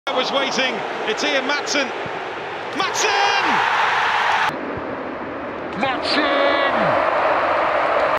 Ian Maatsen scores a corker and Vik can't believe it
Viktor Gyökeres can't believe whats going on at Coventry City